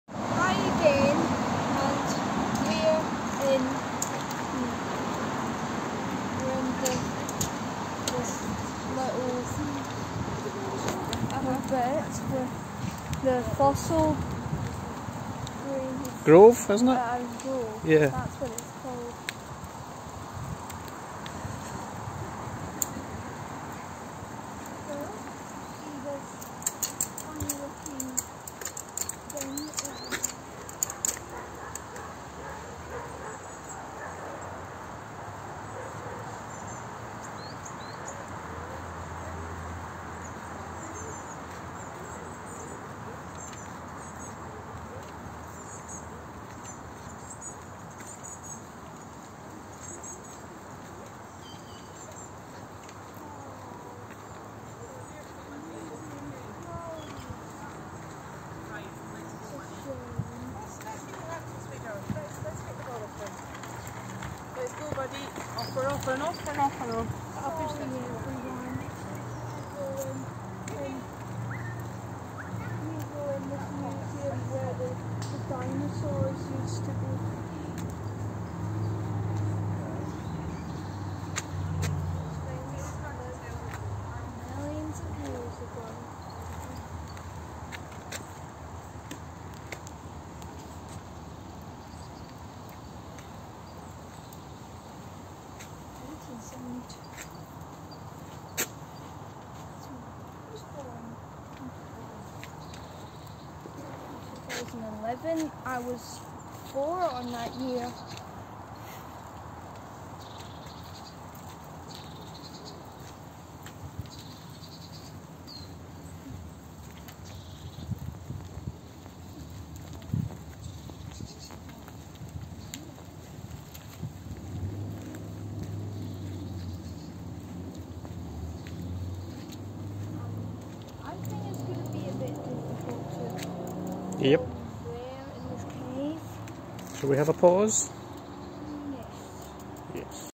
I'm in Fossil Grave
in Fossil Grave there is museum but it's closed 😥 we did that years ago